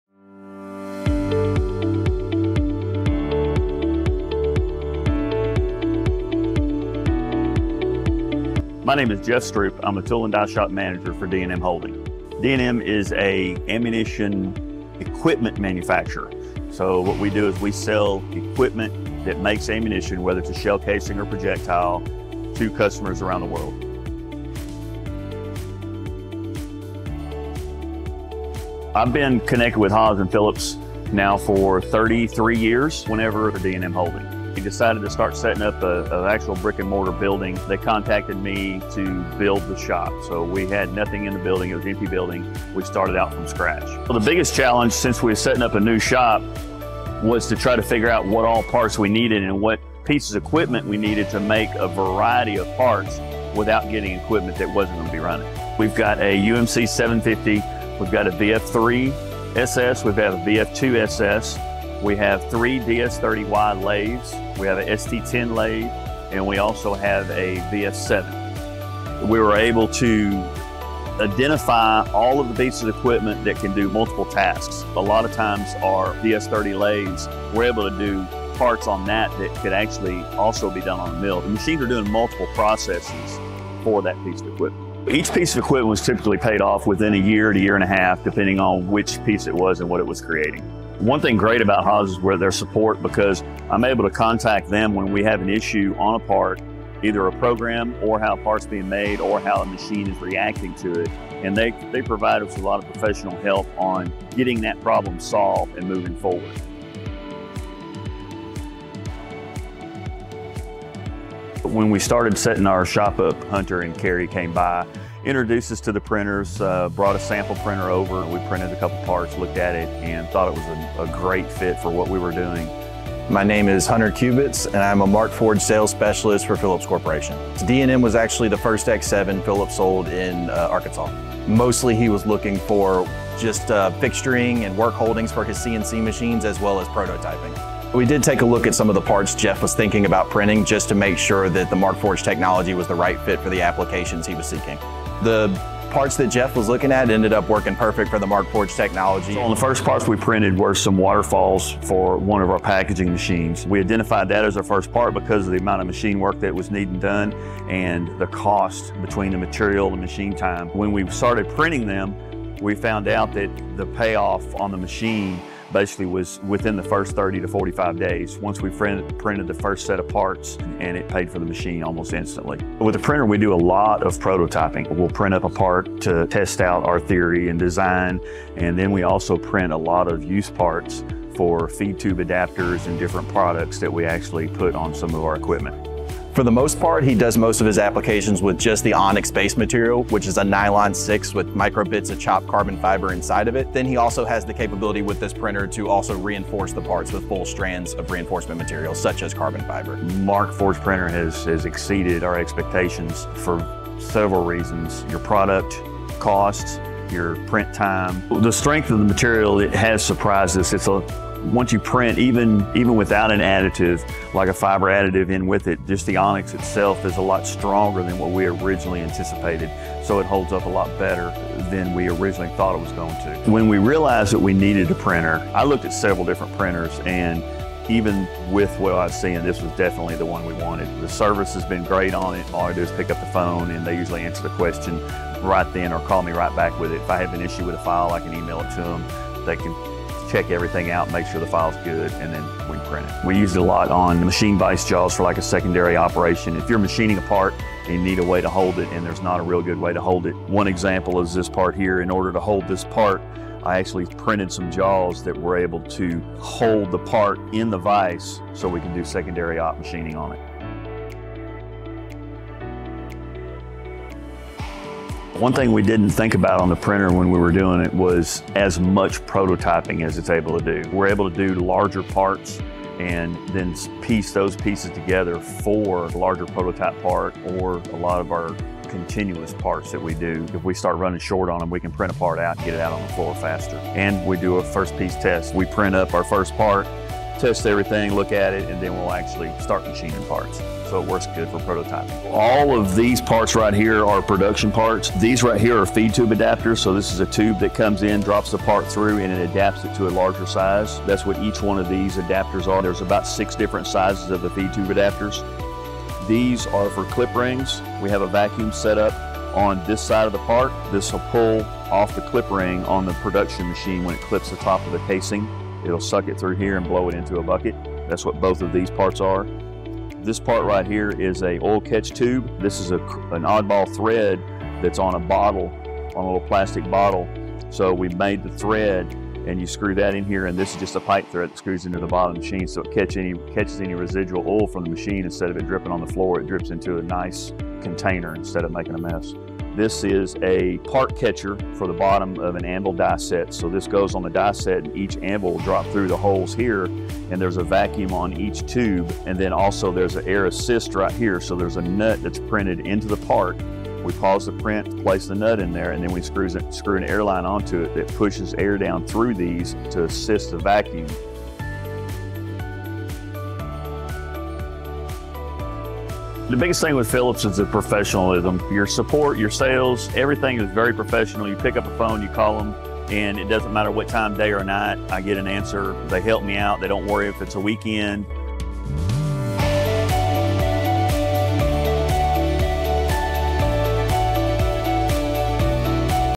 Learn how D&M Holding incorporated 3D Printing and saw ROI in less than 45 days! 💥
Embark on a journey of innovation and rapid ROI with Jeff Stroope as we delve into his inspiring story at D&M Holding. Witness the transformation of an empty space into a thriving ammunition equipment manufacturing hub, driven by the combined forces of 3D printing and CNC machining. Discover how Phillips Corporation's collaboration with Markforged elevated efficiency and achieved astonishing ROI in less than 45 days. Get ready to witness the fusion of determination, advanced tech, and exceptional support that led to an exceptional manufacturing triumph.

Curious about the power of 3D printing, CNC machining, and rapid ROI? Experience Jeff's journey and the revolutionary technology that made it all possible.